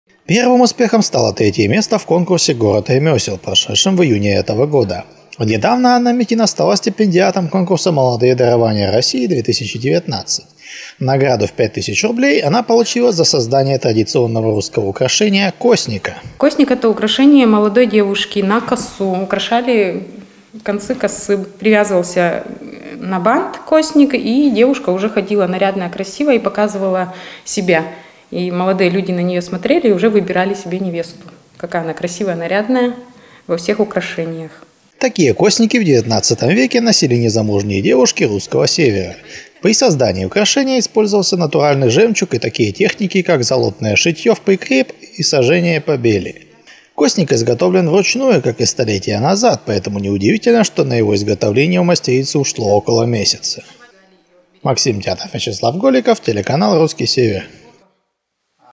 Успехи Анны Митиной
Талант устюжанки Анны Митиной из отдела традиционной народной культуры «Лад» продолжает приносить ей победы. Уже второй раз за текущий год её работы занимают призовые места на конкурсах народного мастерства.